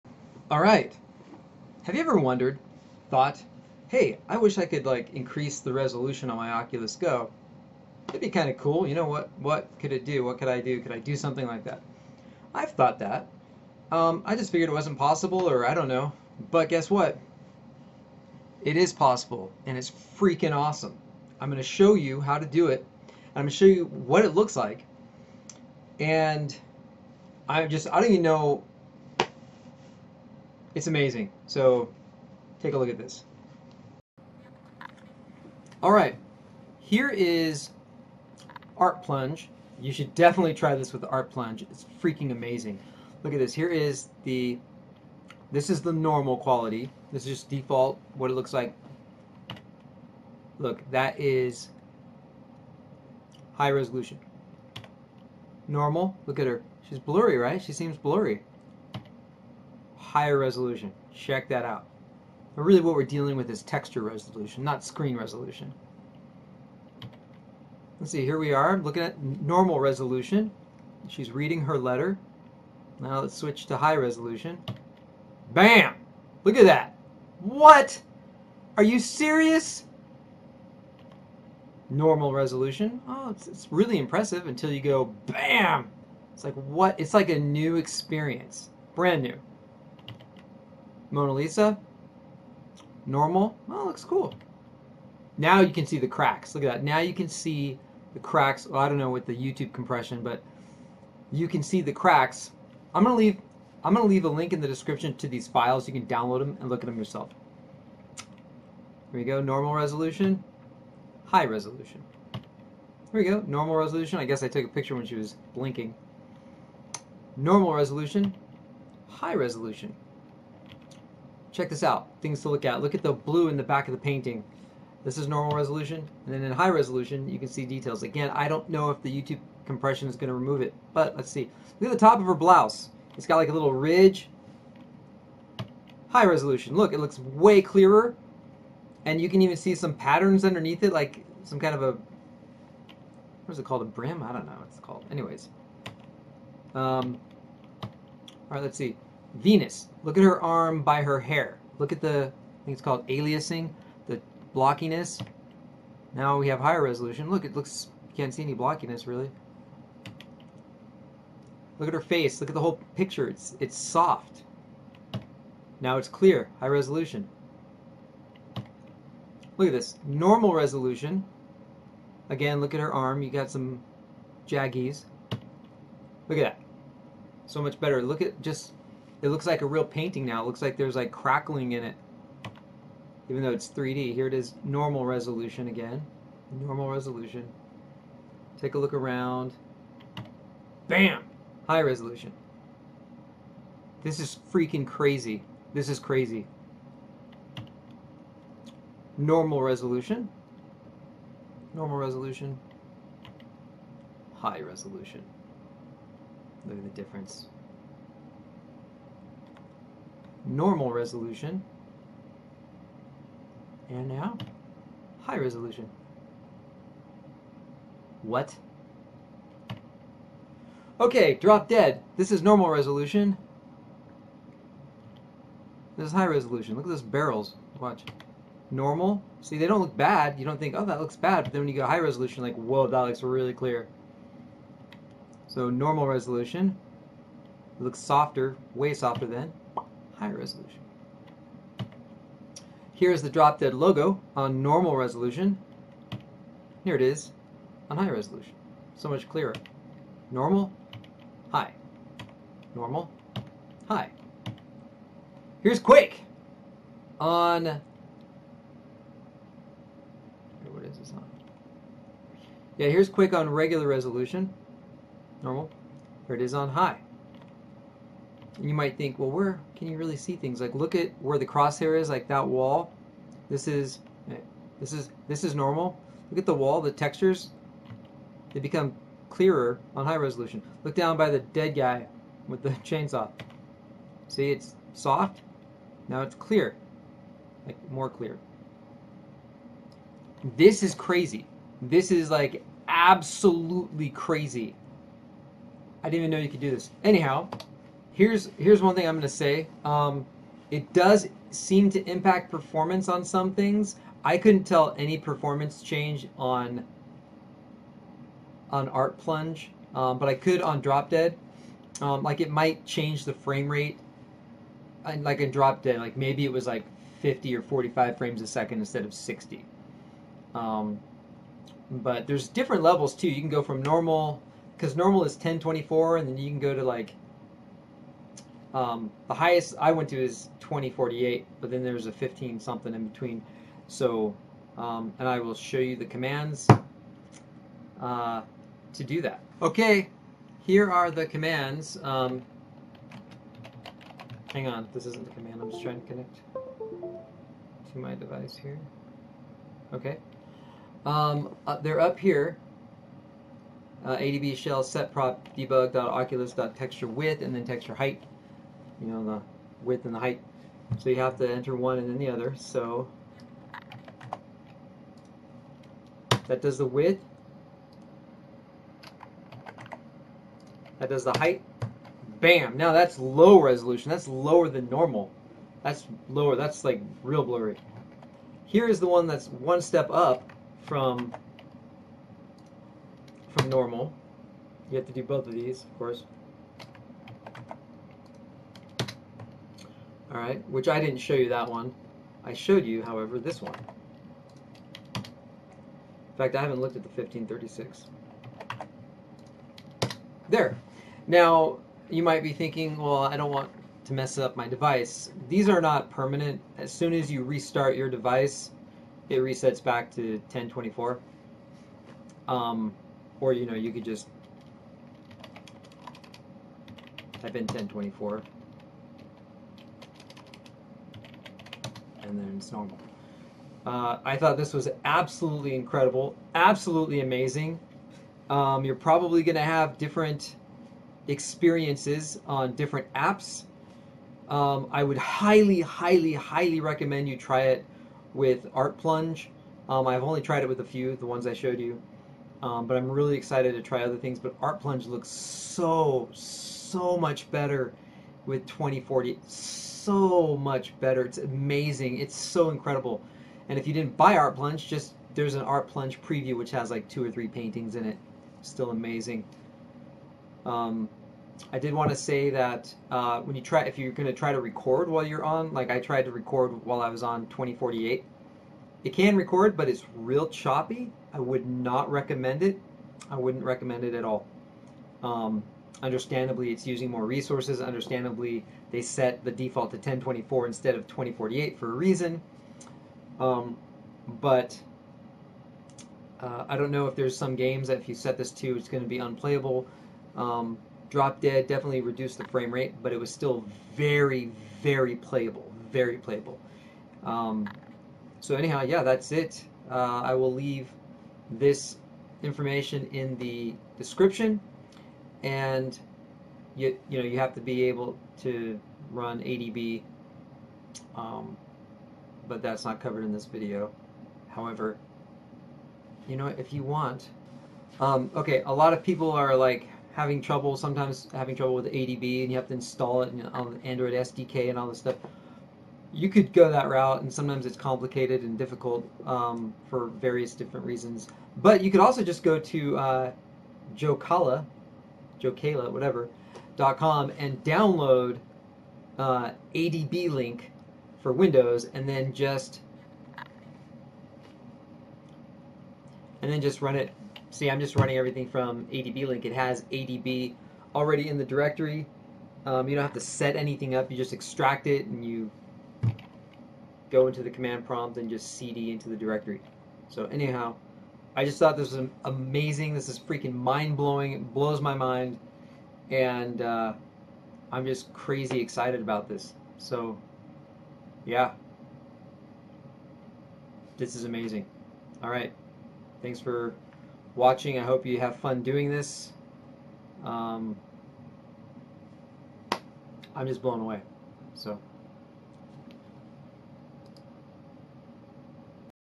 Increase Resolution on the Oculus Go
Get your Oculus Go here:
32GB version:
64GB version:

This video shows you how to increase the TEXTURE resolution on your Oculus Go.

using adb...
Run these 2 commands for LOW resolution
adb shell setprop debug.oculus.textureWidth 768
adb shell setprop debug.oculus.textureHeight 768

Run these 2 commands for NORMAL resolution
adb shell setprop debug.oculus.textureWidth 1024
adb shell setprop debug.oculus.textureHeight 1024

Run these 2 commands for HIGHER resolution
adb shell setprop debug.oculus.textureWidth 1536
adb shell setprop debug.oculus.textureHeight 1536

Run these 2 commands for HIGHEST resolution
adb shell setprop debug.oculus.textureWidth 2048
adb shell setprop debug.oculus.textureHeight 2048

These commands also work with the Oculus Quest